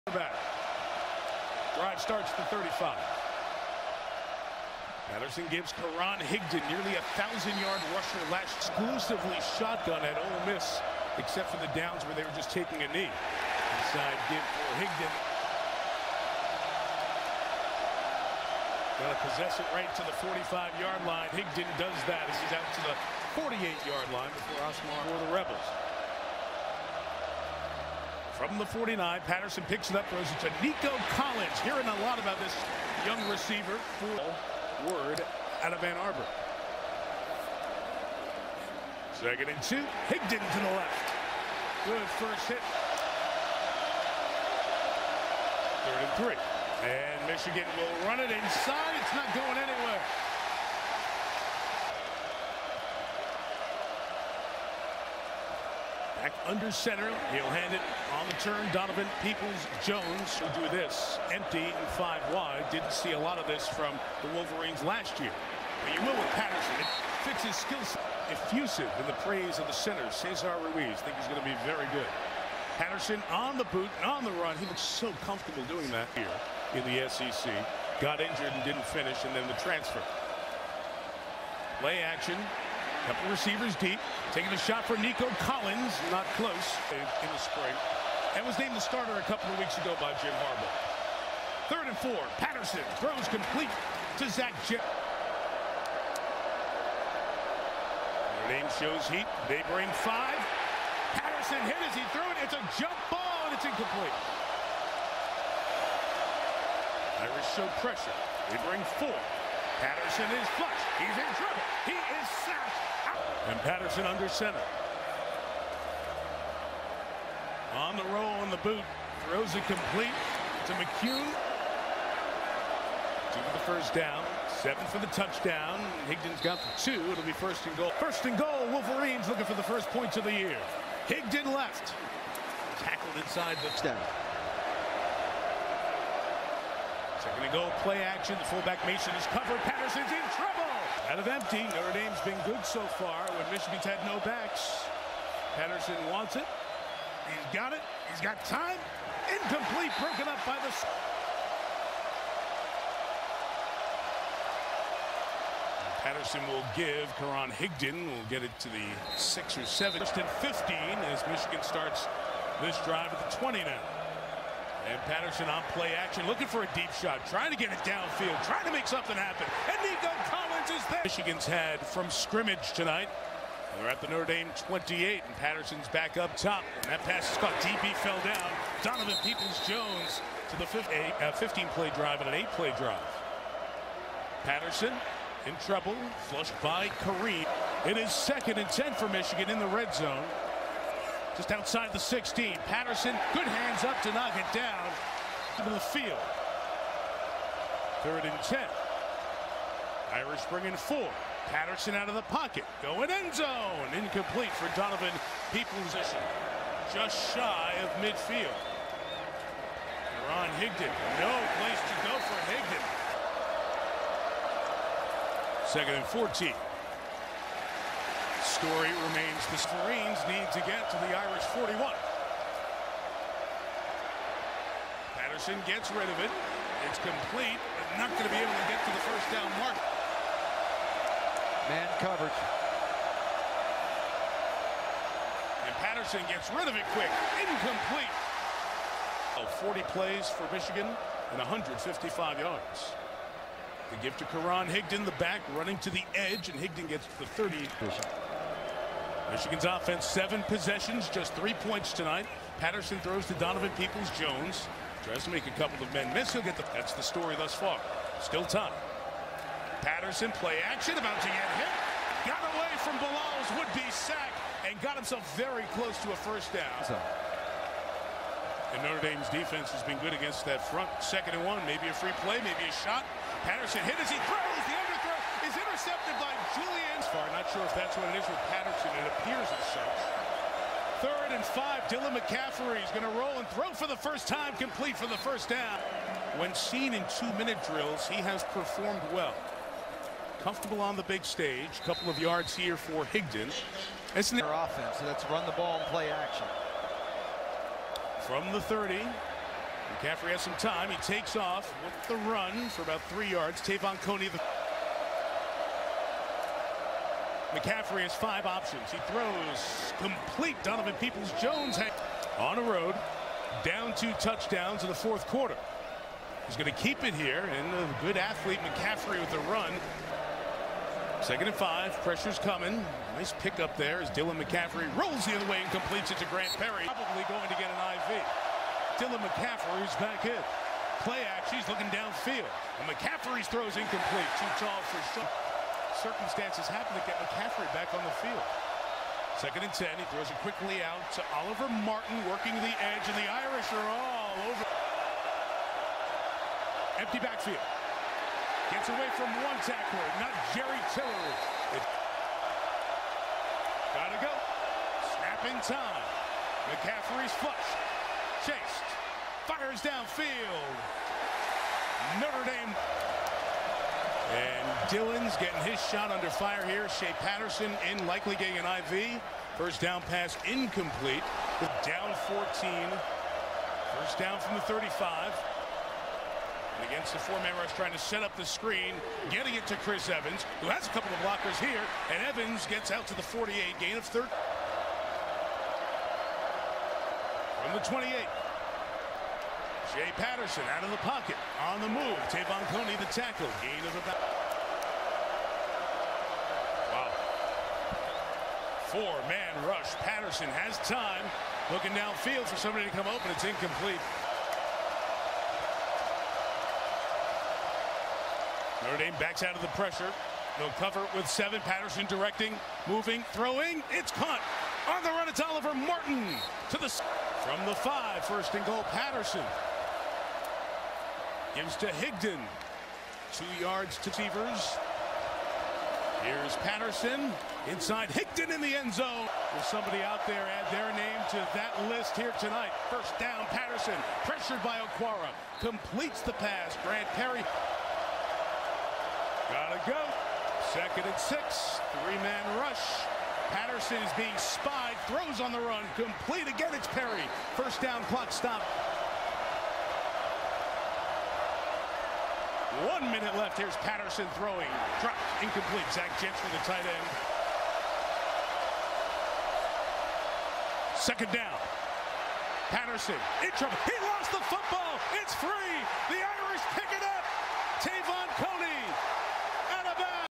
Jerry Tillery (Notre Dame DL) vs. Michigan 2018
Notre Dame DL Jerry Tillery posts 3 tackles, a tackle for a loss, and a sack against the Michigan Wolverines. 9/1/18.

Notre Dame wins 24-17.